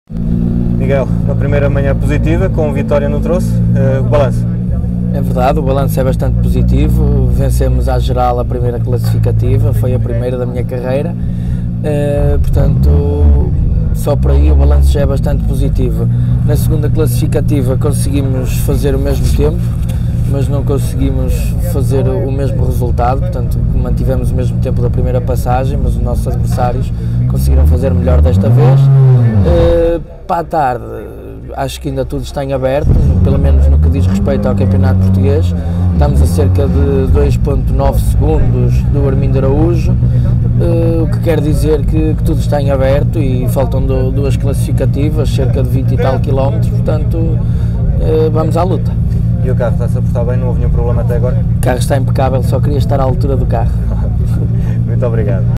CPR, Miguel Correia: “Balanço muito positivo, vencemos a nossa 1ªPEC à geral…”
Declarações de Miguel Correia no final da manhã do segundo dia do Rali Terras d'Aboboreira.